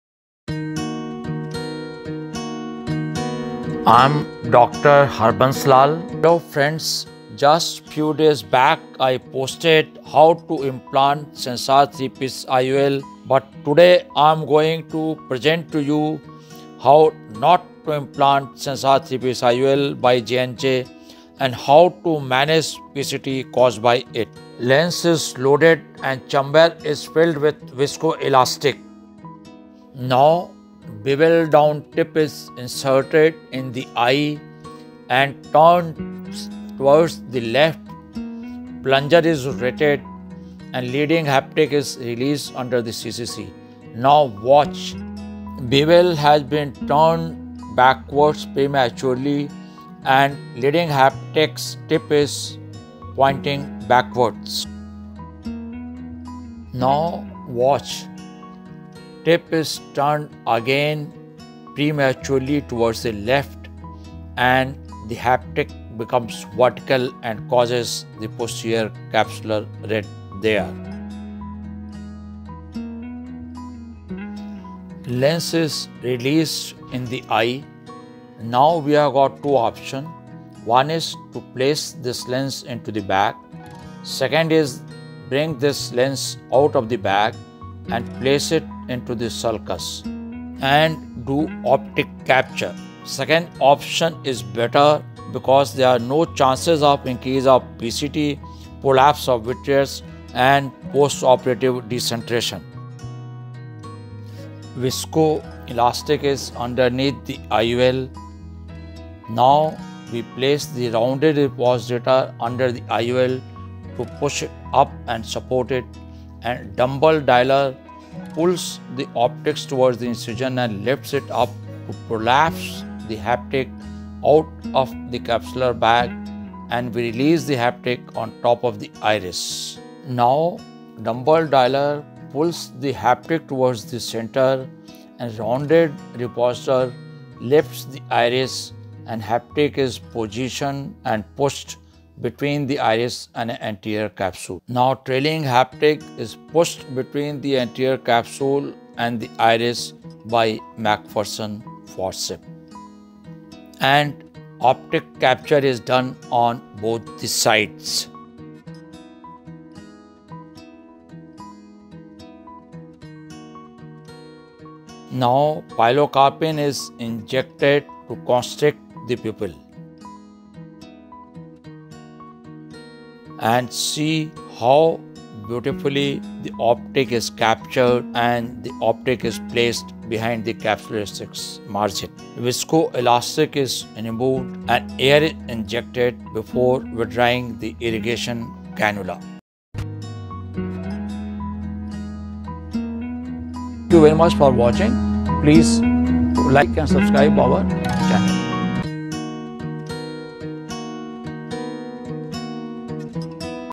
HOW NOT TO IMPLANT SENSAR '3 ' PIECE IOL - DR. HARBANSH LAL
Phaco Tips & Tricks #14 by Dr. Harbansh Lal
HOW NOT TO IMPLANT SENSAR '3 ' PIECE IOL BY JOHNSON & JOHNSON